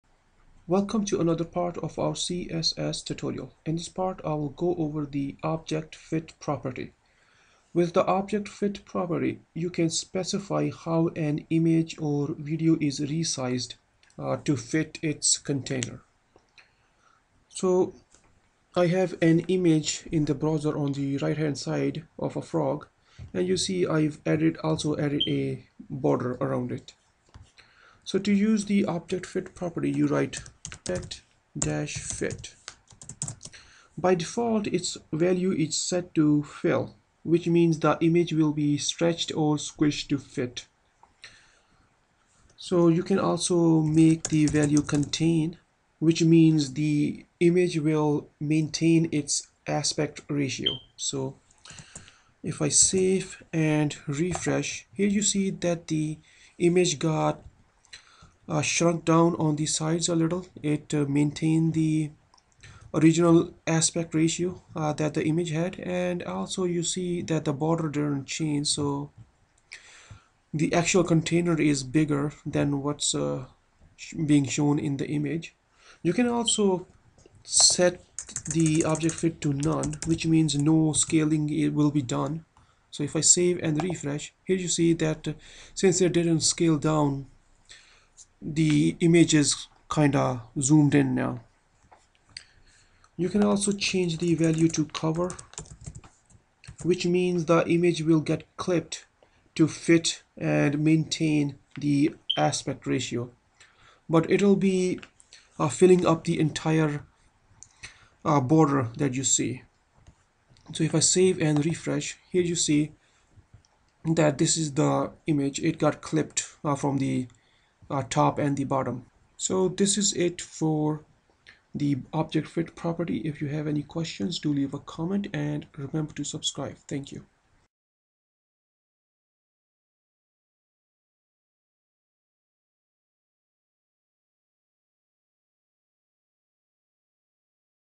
CSS Tutorial 38 - object fit property
In this CSS tutorial I go over the object-fit property. With the object-fit property, you can specify how an image or video is resized to fit its container.

By default, the object-fit property is set to fill, which means the image will be stretched or squished to fit.

By setting object-fit property to contain, it will maintain the aspect ratio, thus shrinking the image size as per the container.

By setting object-fit property to none, it will not shrink down the image to fit container. Only what can fit in container will get shown, rest will be trimmed.

By setting object-fit property to cover, it will maintain the aspect ratio and will be clipped if necessary.